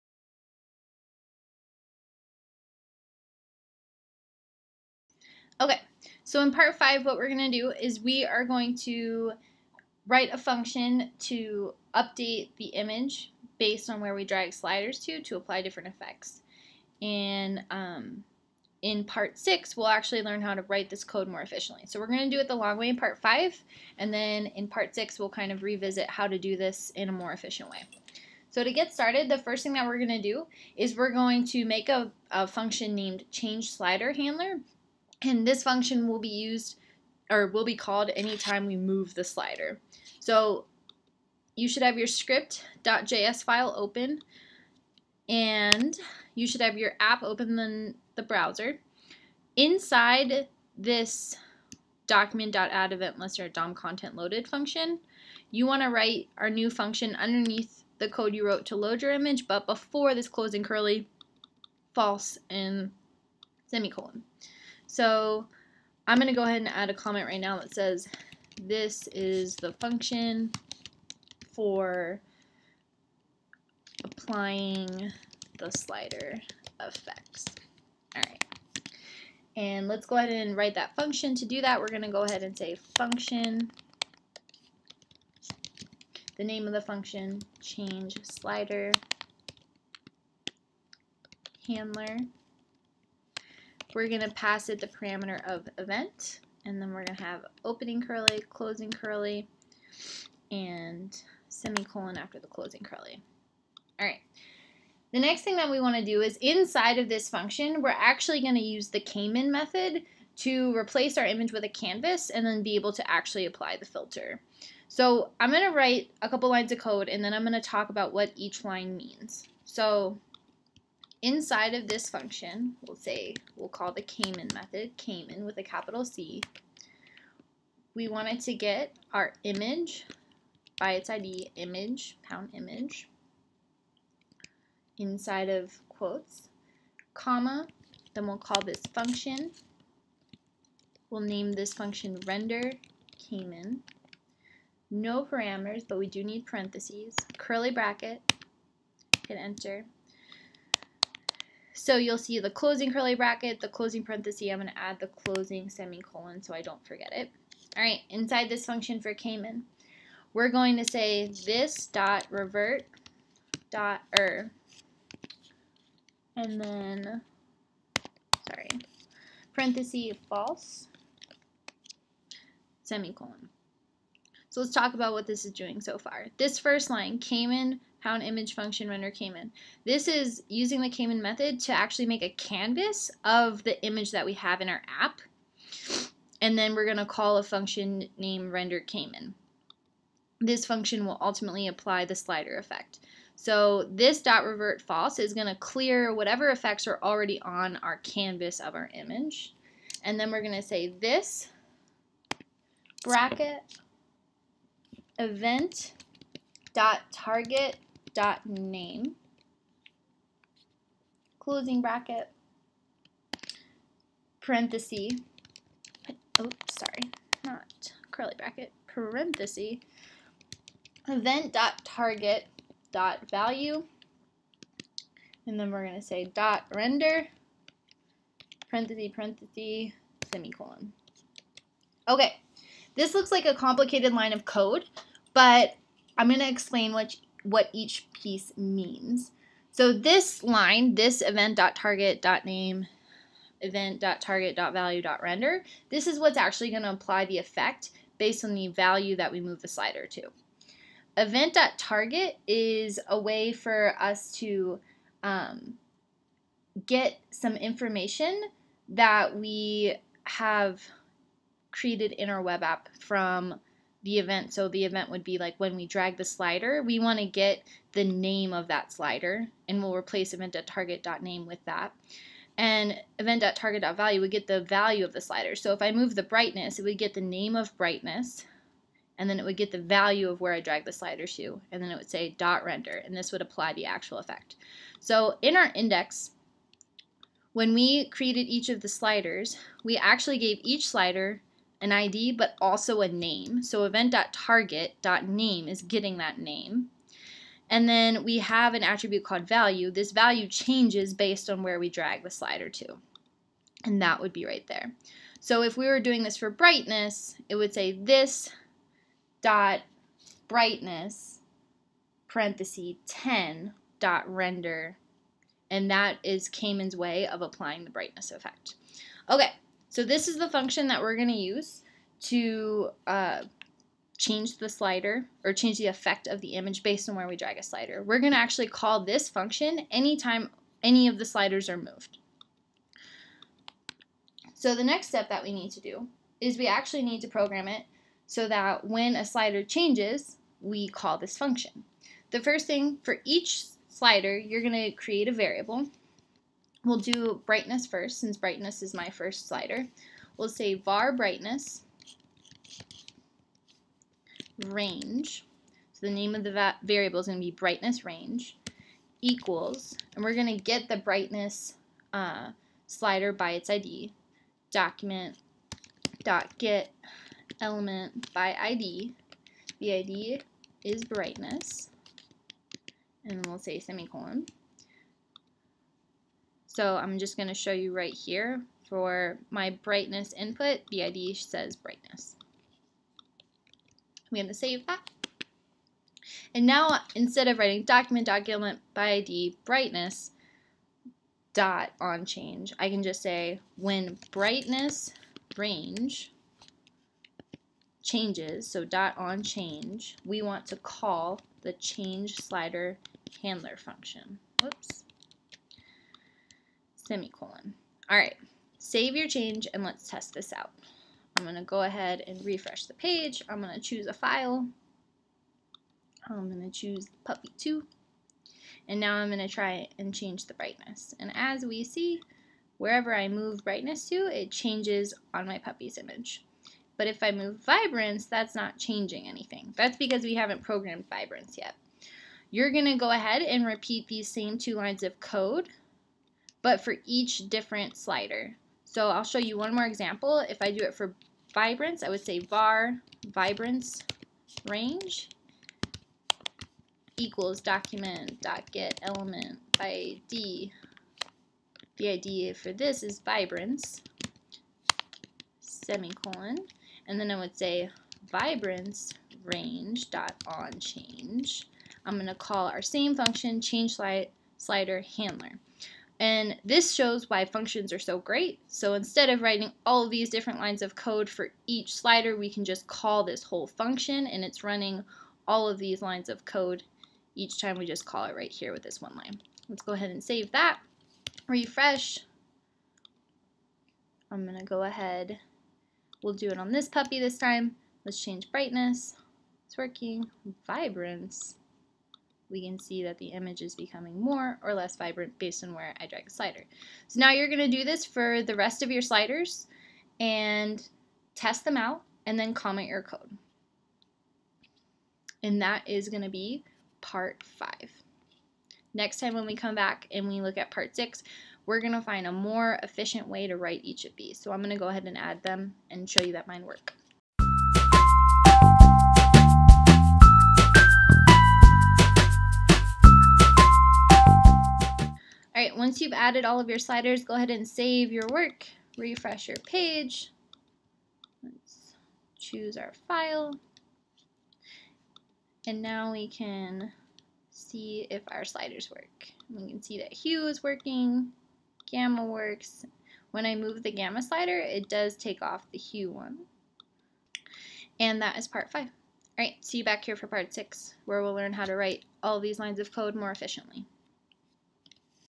Photo Editor Web App Part 5
In this tutorial you will use the Caman function, functions and variables to apply slider effects on the image when the sliders are moved.